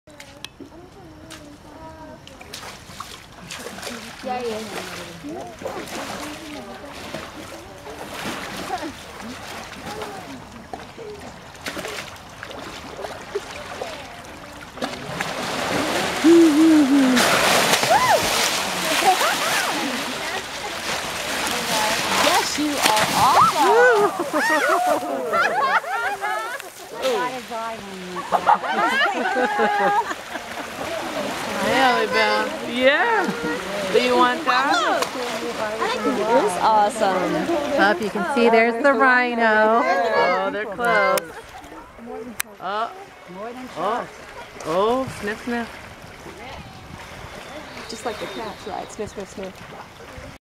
Meeting the neighbors San Francisco hippo and rhino meet for the first time
hippo and rhino meet for the first time